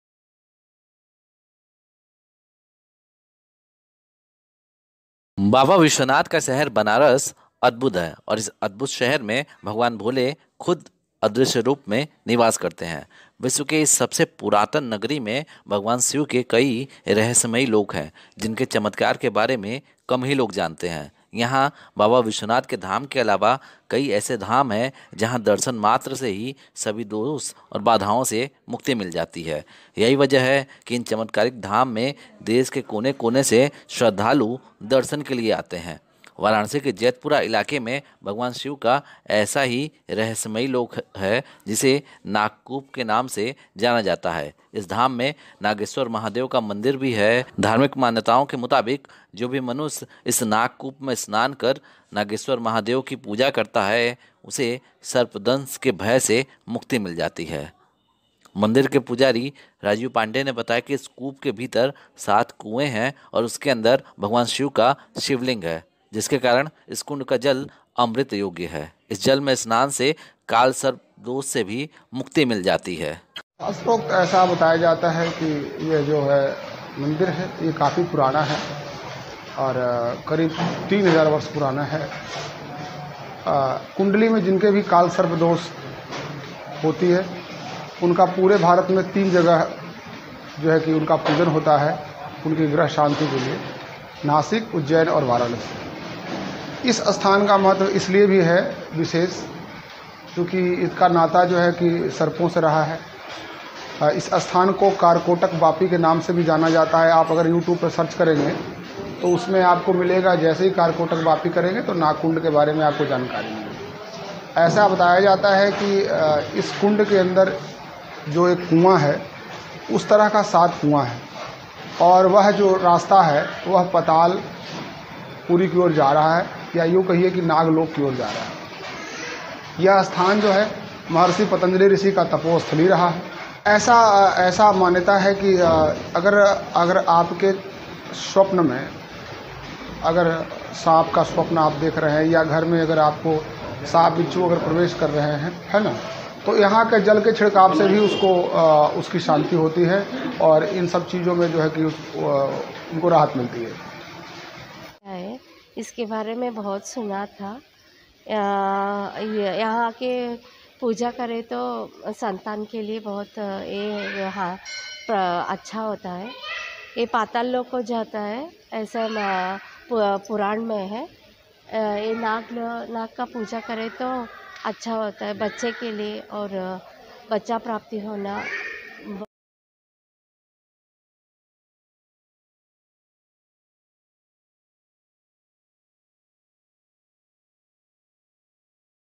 Mahashivratri 2023: काशी में भगवान शिव का रहस्यमयी लोक, जानिए 3 हजार साल पुराना रहस्य
Mahashivratri 2023: वाराणसी (Varanasi) के जैतपुरा इलाके में भगवान शिव का ऐसा ही रहस्यमयी लोक गई जिसे नागकूप के नाम से जाना जाता है. इस धाम में नागेश्वर महादेव का मंदिर भी है, धार्मिक मान्यताओं के मुताबिक जो भी मनुष्य इस नागकूप में स्नान कर नागेश्वर